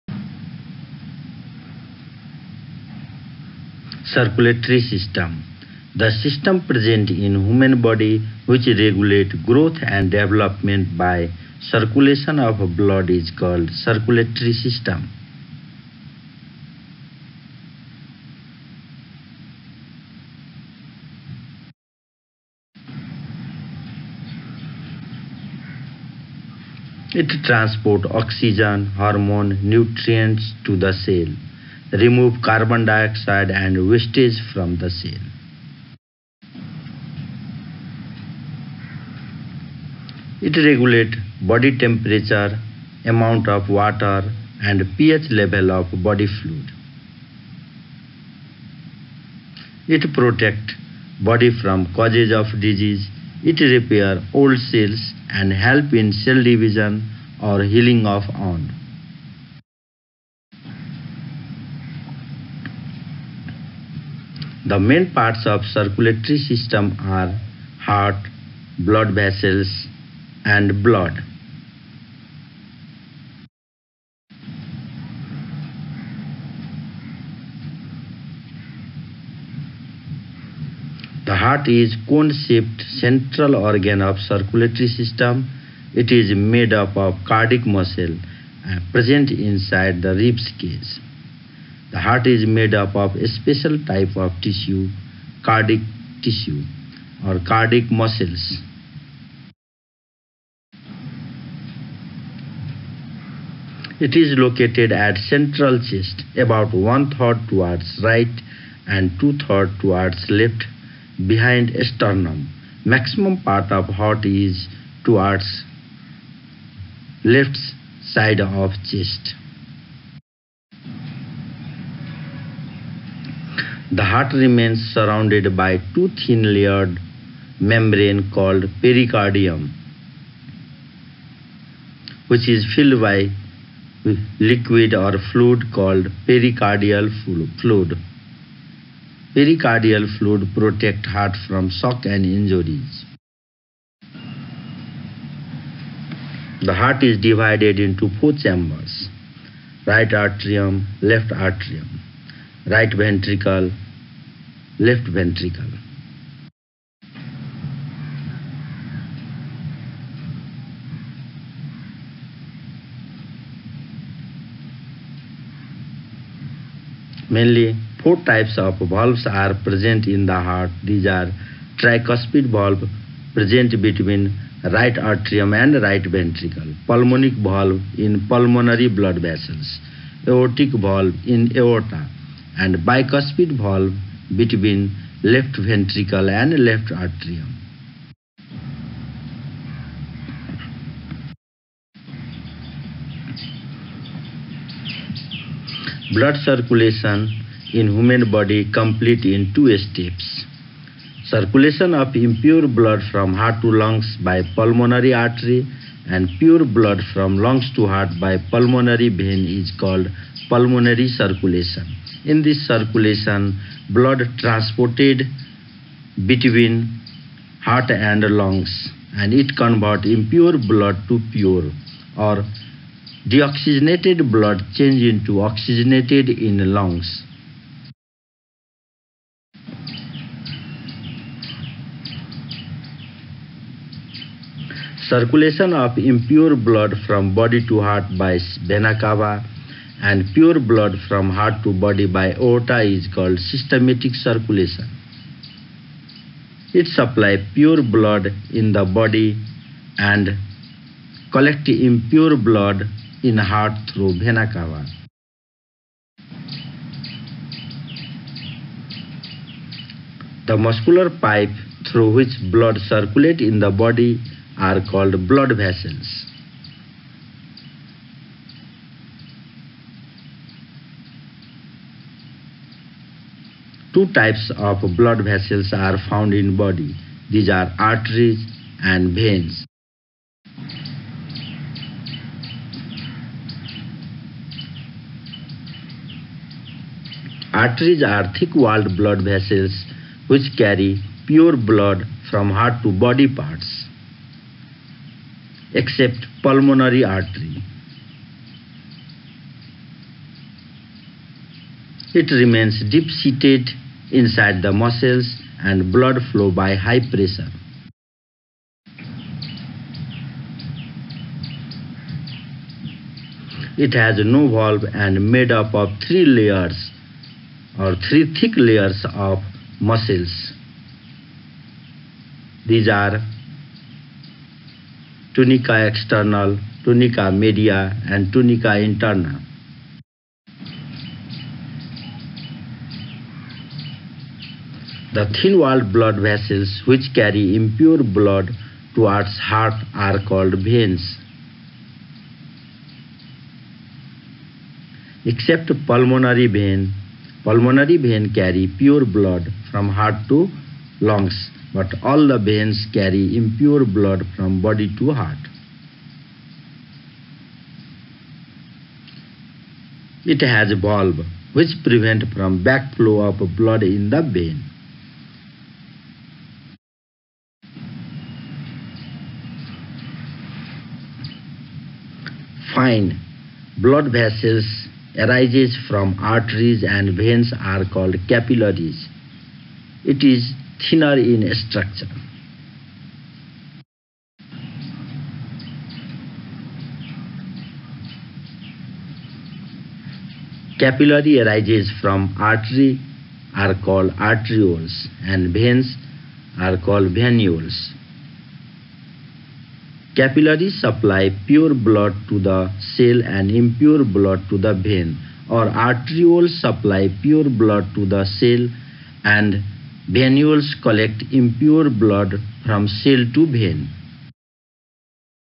Blood circulation in human body // Circulatory system Class 10
Blood circulation in human body.
Internal structure of heart.
Arteries and Veins.
Pulmonary and systematic circulation.
internal parts of the heart.
Valves present in the heart.
blood pumping system.
capillaries.
blood capillaries.
Class 10 circulatory system